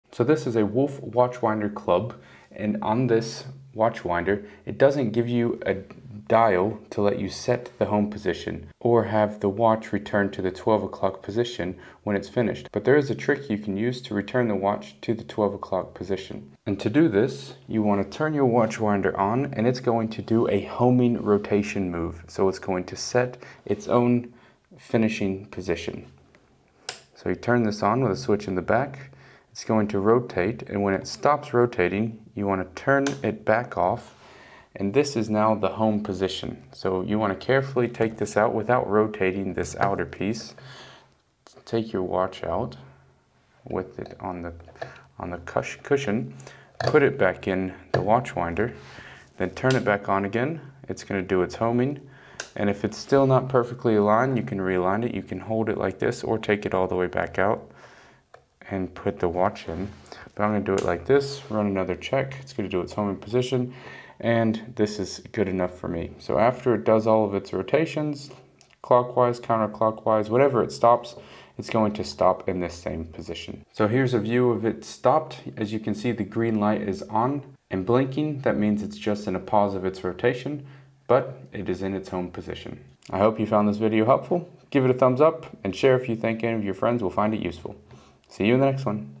Center Wolf Watch Winder | Return to 12 O'clock position
It took me a while to realize this watch winder could actually return itself to the 12 o'clock position, but it doesn't not it automatically. Here is a quick trick to return the wolf cub watch winder to the 12 O'clock position.